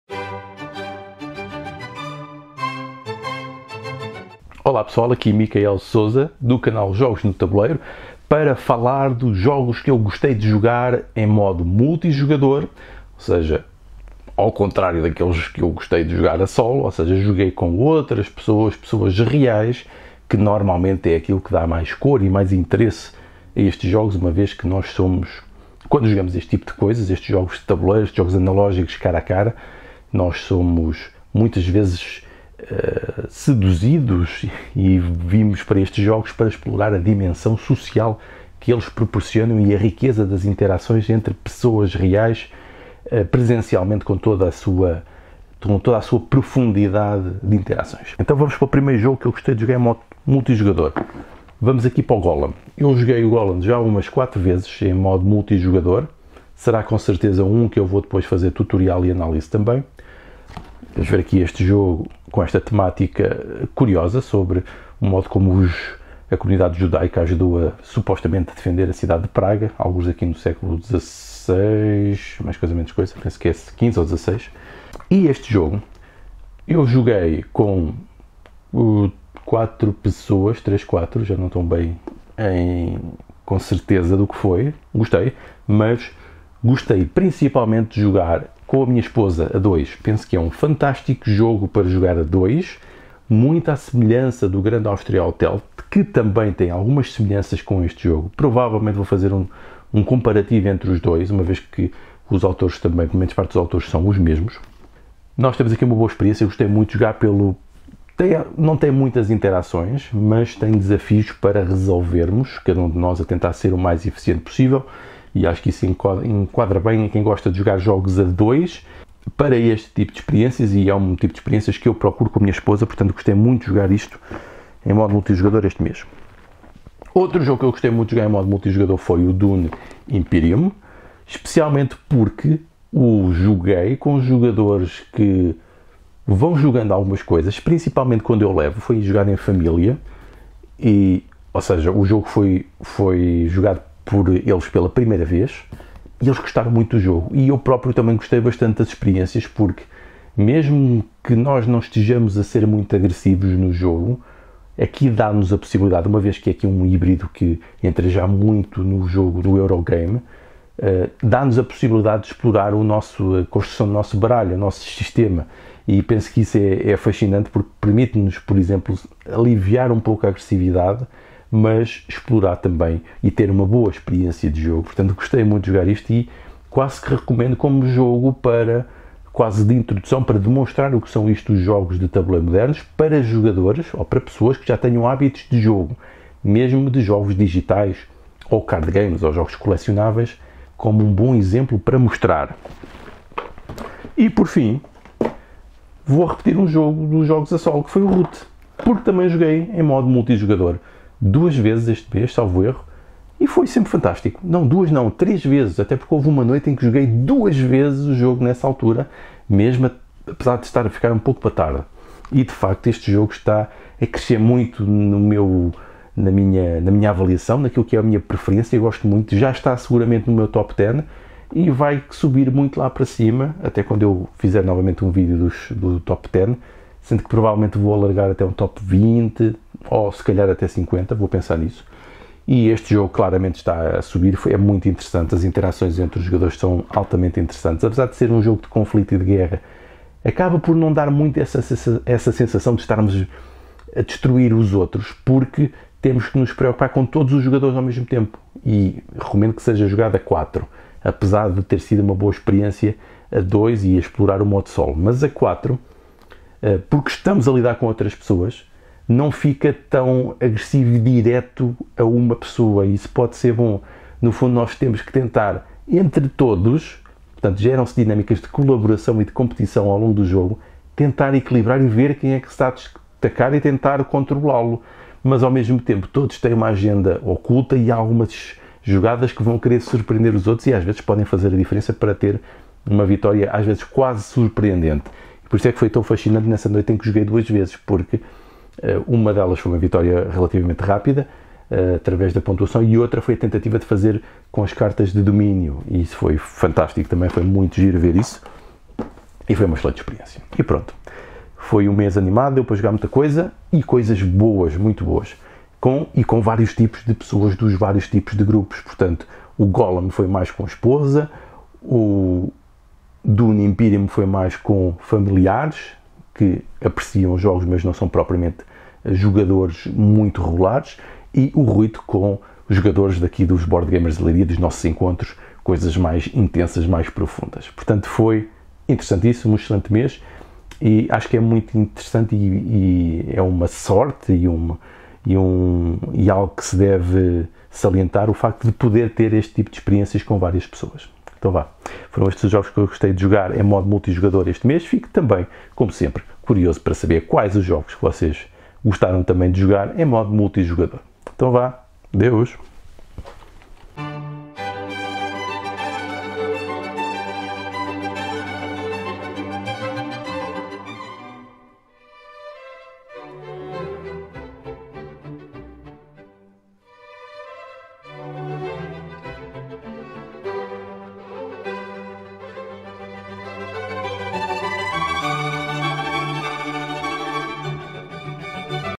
Jogos que gostei de jogar em multijogador em Abril de 2022
Estes foram os jogos que gostei de jogar com mais jogadores, em modo multijogador, em Abril de 2022. Quais foram os vossos?